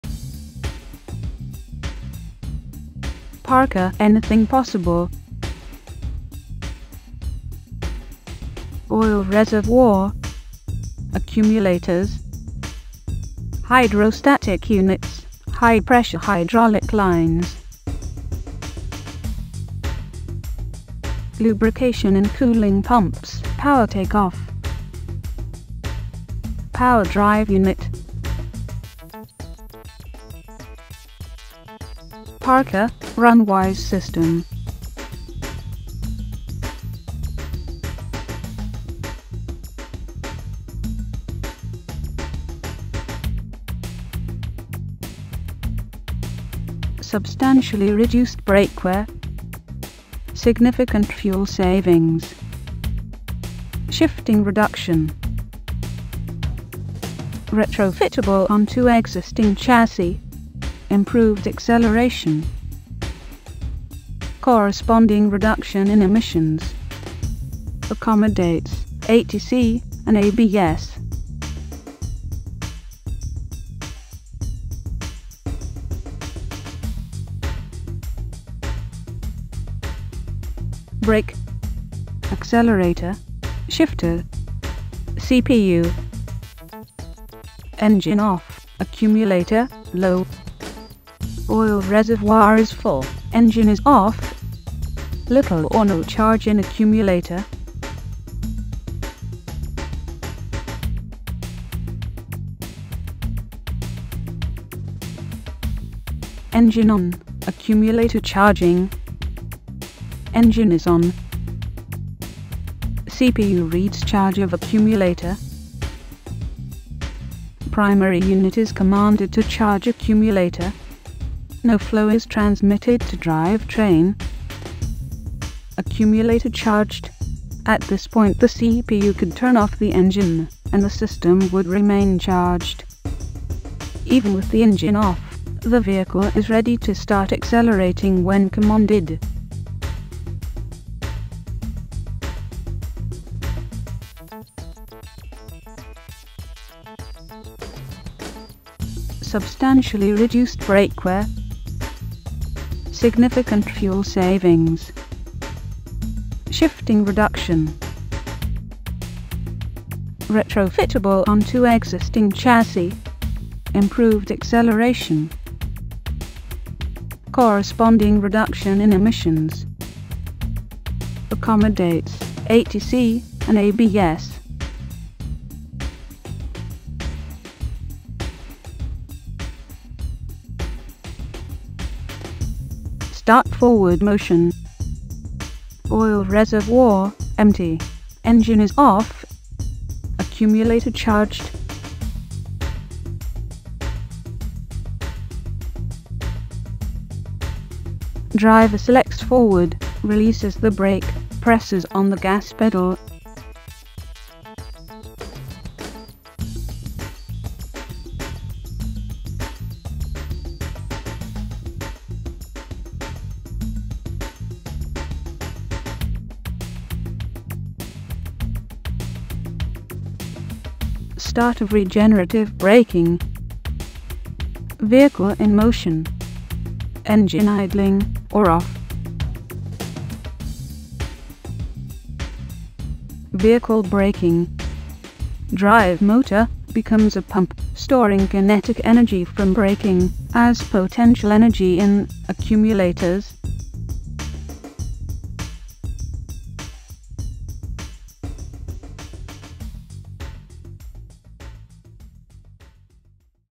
Animated System Infographic
System overview, animated walkthrough, with synthetic voiceover. Used for tradeshow promotion, HD video and website.